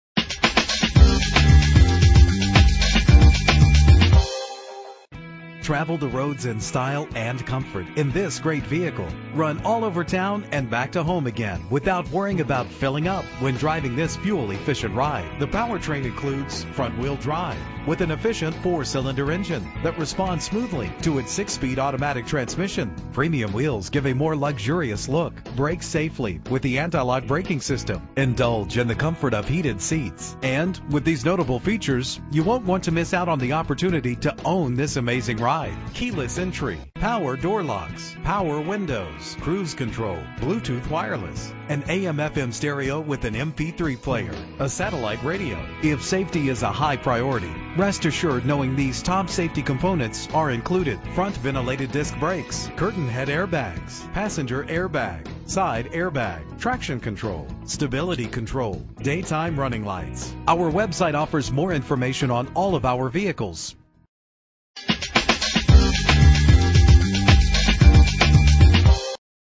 2014 Chevrolet Sonic - Owosso MI
Year: 2014
Make: Chevrolet
Model: Sonic
Trim: RS
Engine: 1.4 liter inline 4 cylinder 16 valve
Transmission: 6-Speed Automatic
Color: Ice
Mileage:
Address:
1500 E. Main St.